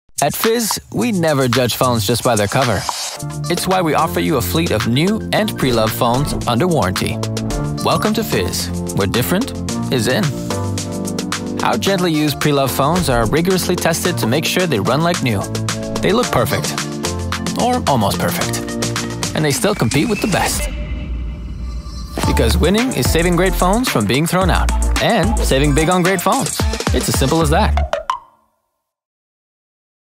Fizz Mobile & Internet - Device - Did you know - 30s
Fizz offers a fleet of new and Preloved phones under warranty. Find the perfect phone for you.